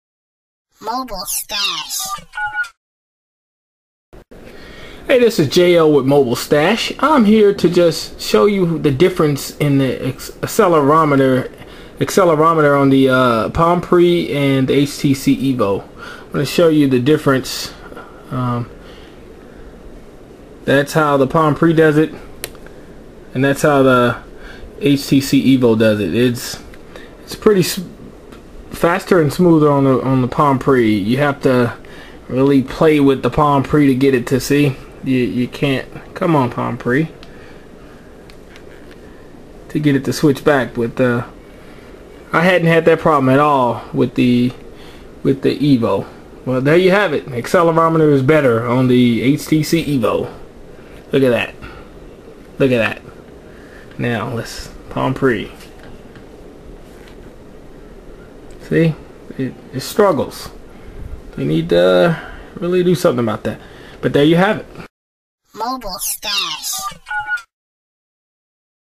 Sprint HTC EVO vs Palm Pre - Accelerometer Comparison
This is a comparison between the HTC EVO and the Palm Pre.  The EVO wins this one...